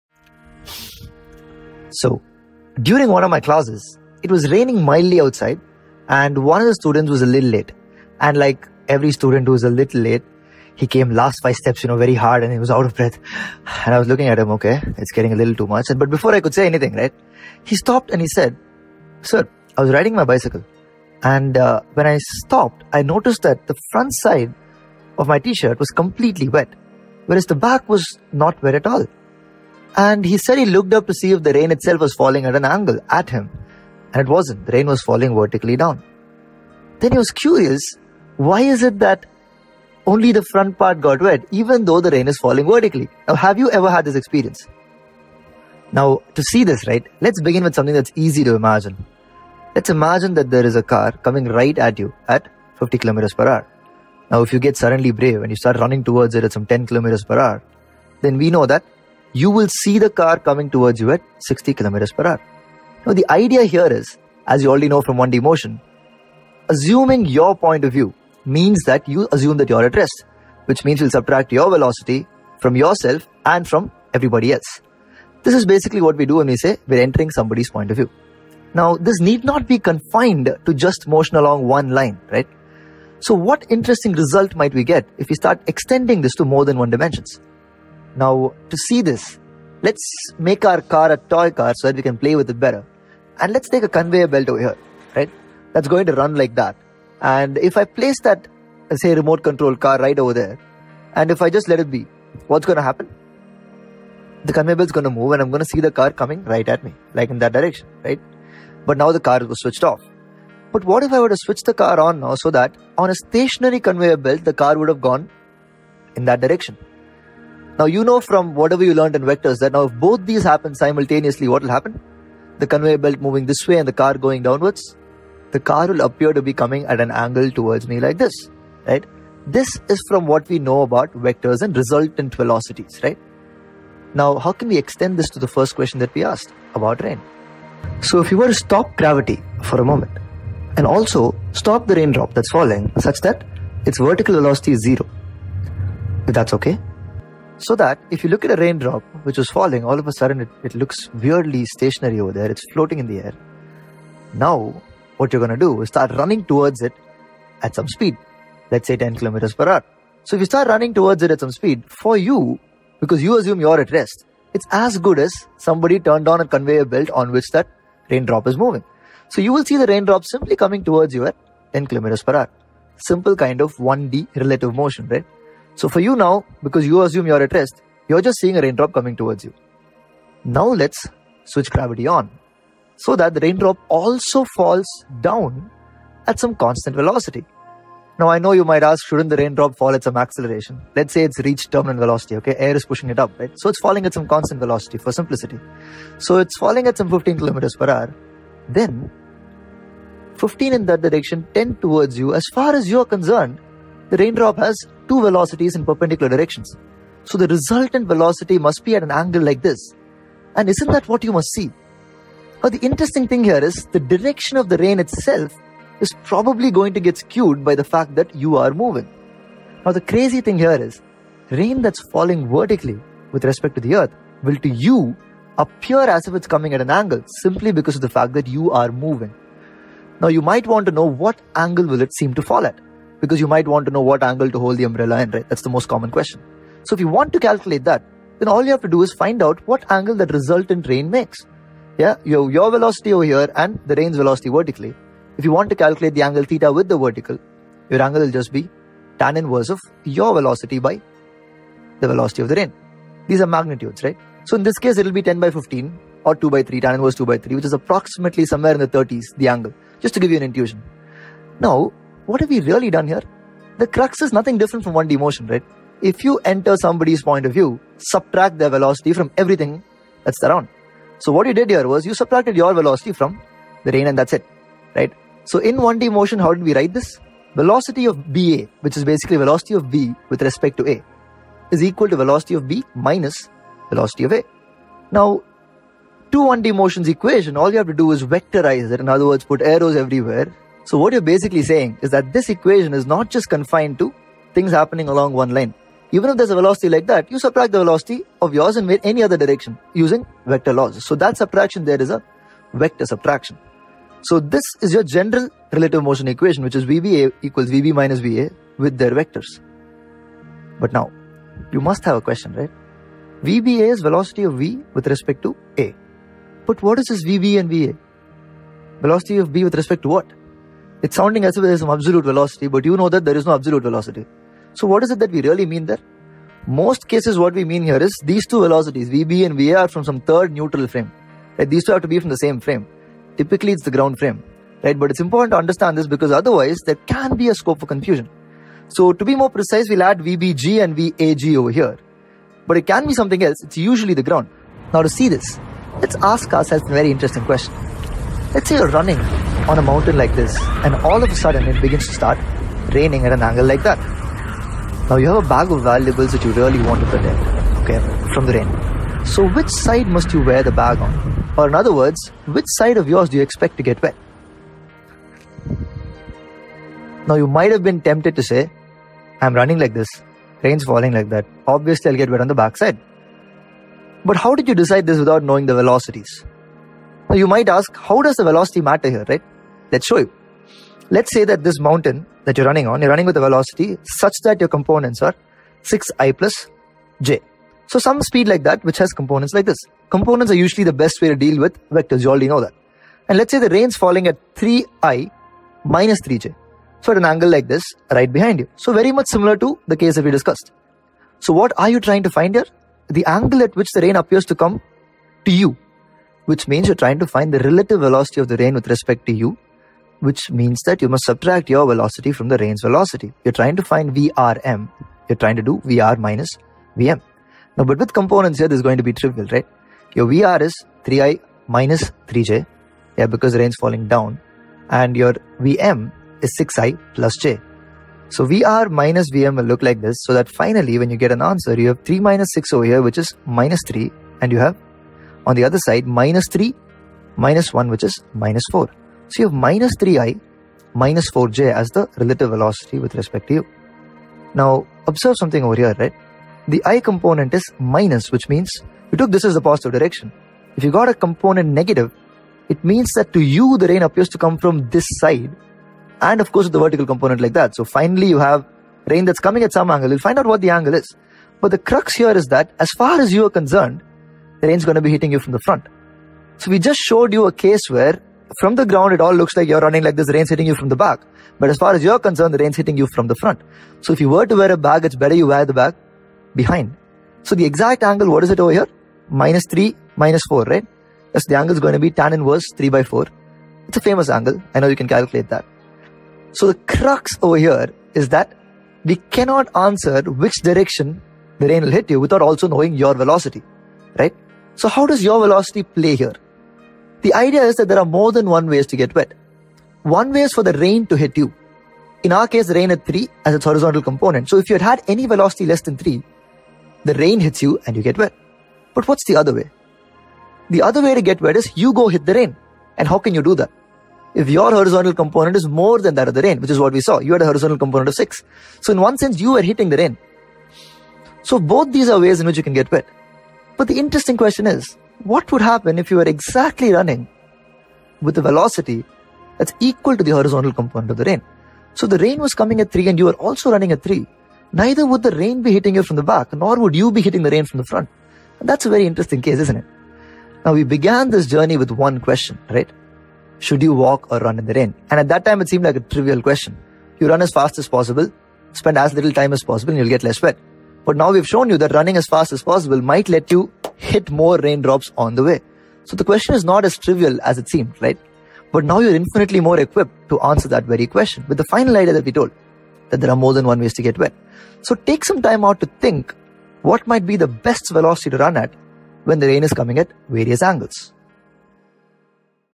Motion in Two Dimension | Concepts visualised | Relative Motion | NEET l Aanand Sir | BYJU'S NEET
The relative motion velocity refers to an object's velocity which is relative to some other object that might be stationary, moving with the same velocity, or moving with some other velocity. After watching this video you will be able to explain that why riding a bicycle you tend to get wet from front while its raining.

Keep Learning!